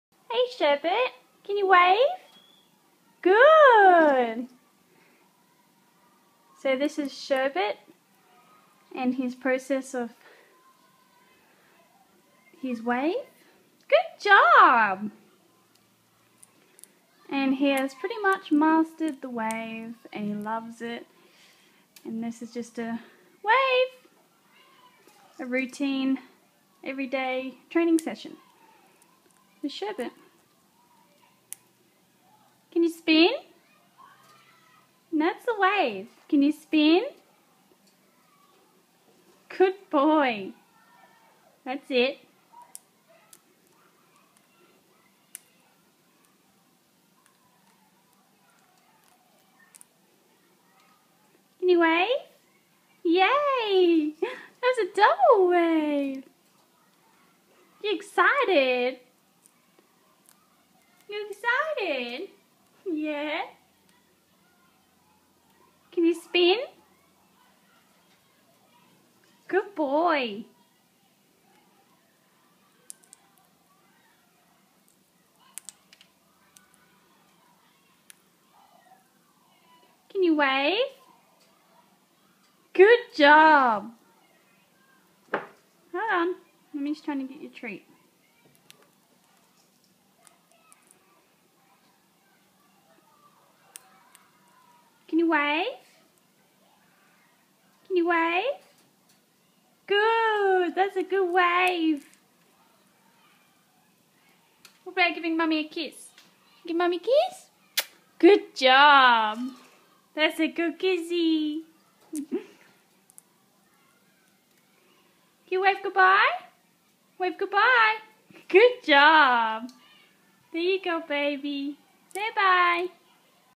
IRN Mastering The Wave!
Indian Ringneck mastering the wave and daily training session.
Sherbert picked up this wave trick in 4 weeks, the spin trick you see in this video is his first trick he learned and took about 3 weeks to learn.
Smarty pants :)